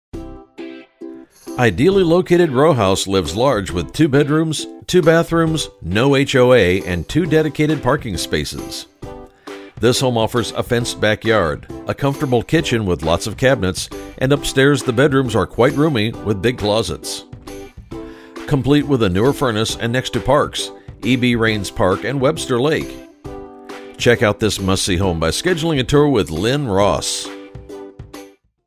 Lynne Ross presents 11672 Lincoln Street Northglenn, CO | ColdwellBankerHomes.com
11672 Lincoln Street, Northglenn, CO

Ideally located Rowhouse lives large with two bedrooms and two bathrooms. And the best part: NO HOA! Two dedicated parking spaces located directly in front of the home. Fenced, spacious backyard allows a great place for outdoor living. Comfortable kitchen with lots of cabinets and plenty of room for